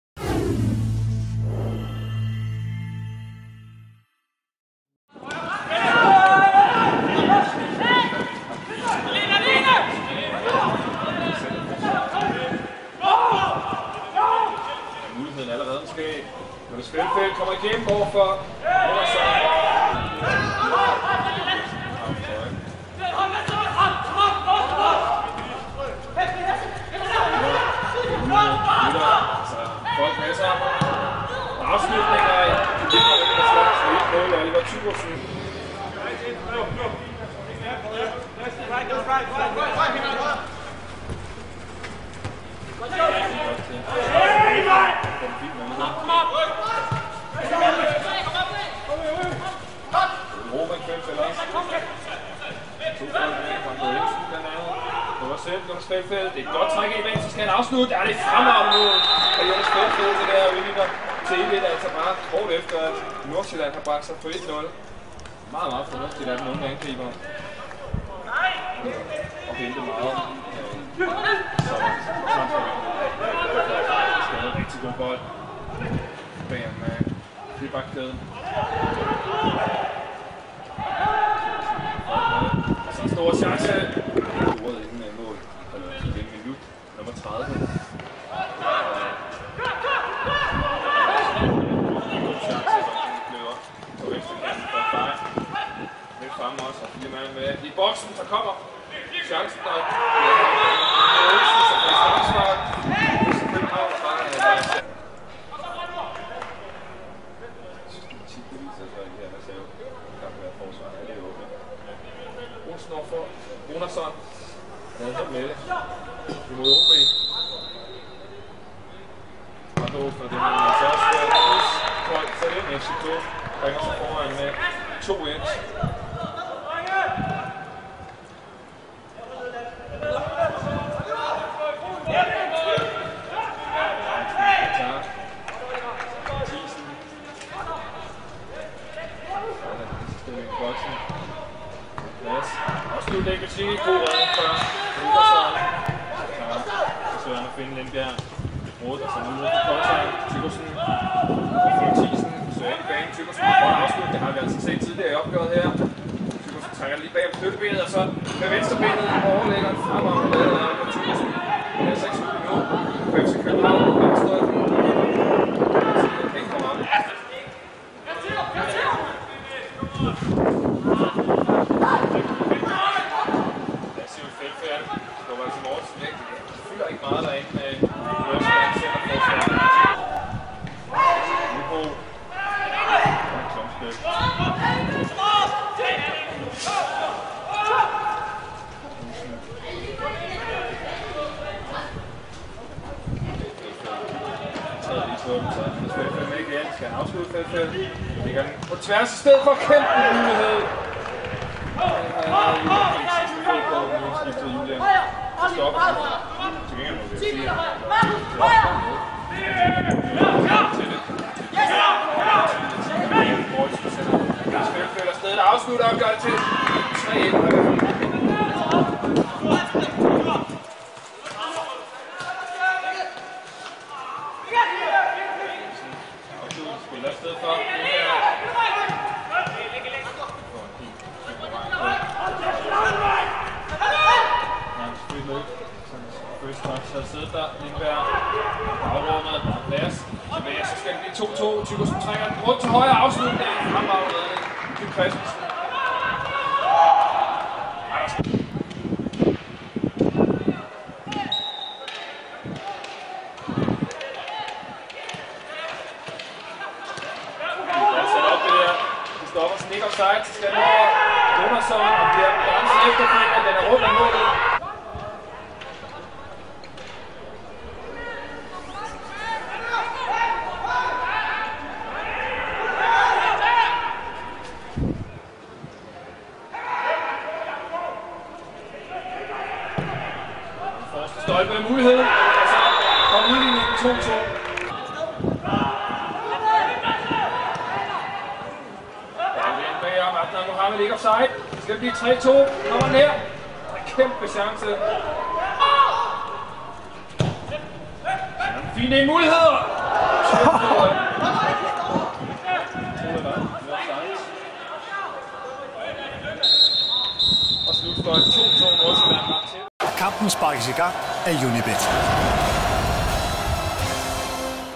Highlights: FCN 2-2 FCK (Reservehold)
Yones Felfel og Brandur Olsen var på pletten, da løvernes reserver mandag eftermiddag spillede 2-2 på Farum Park mod FC Nordsjælland. Se højdepunkterne fra kampen her - vi beklager den forringede billedkvalitet.